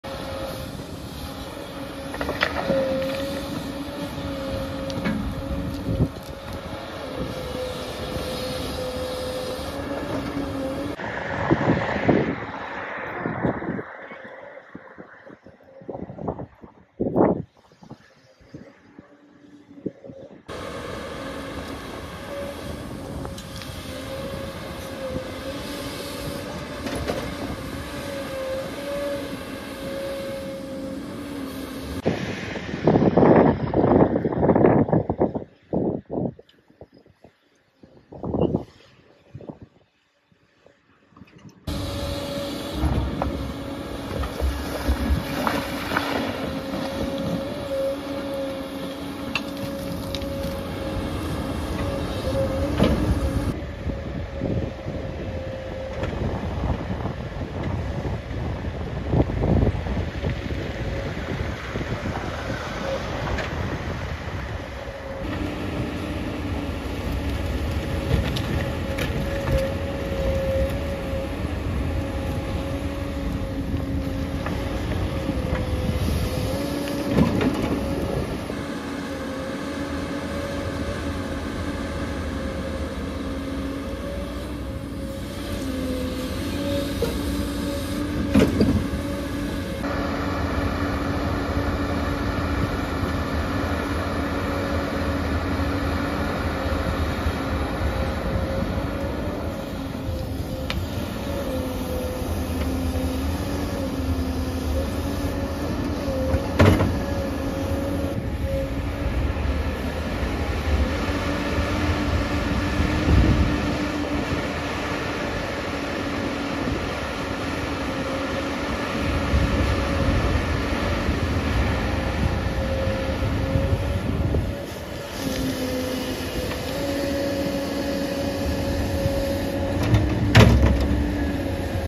Big Digger Moving Dirt | Mighty Machines for Kids | Without Music
Big Digger moving dirt on a construction site. For kids.